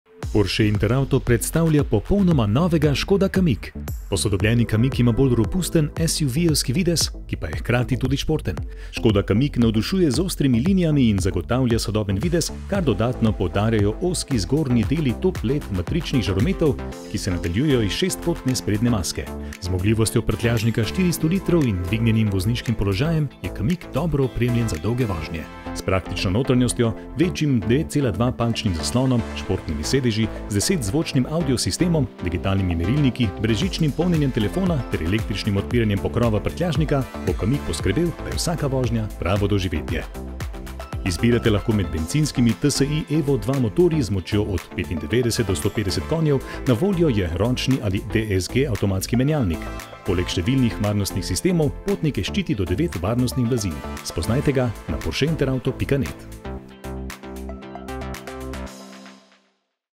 NOVI ŠKODA KAMIQ 2024 - NAROČITE PRI PORSCHE INTER AUTO
Novo v salonih Porsche Inter Auto Slovenija - predstavljamo vam povsem novi Škoda Kamiq 2024! Še bolj terenski SUV kot kdajkoli prej z najnovejšo tehnologijo.

Doživite ga v živo pri:
- Porsche Verovškova
- Porsche Koper
- Porsche Maribor

Novi Škoda Kamiq - še več terenskega videza
Novi Škoda Kamiq 2024 vas bo prepričal s številnimi novostmi. Veliko novosti je prevzel iz drugih športnih terenecev znamke, kot recimo Škoda Kodiaq in Škoda Karoq. Pri Kamiqu je spremenjen tudi sprednji del, ki zdaj daje še bolj samozavesten vtis - nova odbijača z izrazitima difuzorjema pa mu dajeta bolj robusten SUV-jevski videz, ki pa je hkrati tudi športen.

Oznake